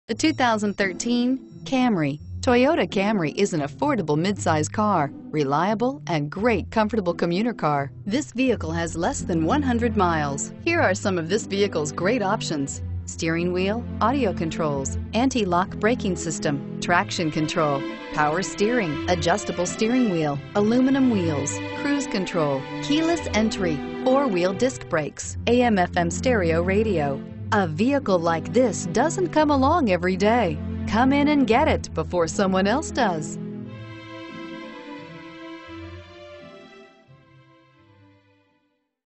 2013 TOYOTA CAMRY Milford, CT D1004
2013 TOYOTA CAMRY Milford, CT
Stock# D1004

Colonial Toyota
470 Boston Post Rd

America's top-selling passenger car for the past ten years, the Toyota Camry receives a major overhaul for 2013, and the seventh-generation of the front-drive sedan retains its midsize status. The 2013 Toyota Camry sedan is a generational redesign that builds on the midsize sedan's historic strengths by complementing its practical nature with an even more modern character and better value at all trim levels. The 2013 Toyota Camry cabin adds both space and contemporary sophistication, highlighted by a leather-covered  layered  dash set off by trim-specific instrumentation and a center cluster inspired by portable music players. Although its external dimensions are virtually unchanged, the 2013 Toyota Camry's fully redrawn bodywork adds an edgier flair. The new sedan sports a bolder front and rear look with more expressive contours and character lines that impart a wider, lower look and a more wedge-like profile. Arguably better in every measurable way, the thoroughly tweaked 2013 Toyota Camry has what it takes to retain its already bountiful buyer base and make an even-stronger case for consideration by anyone seeking a top-rate midsize sedan with either conventional or hybrid power.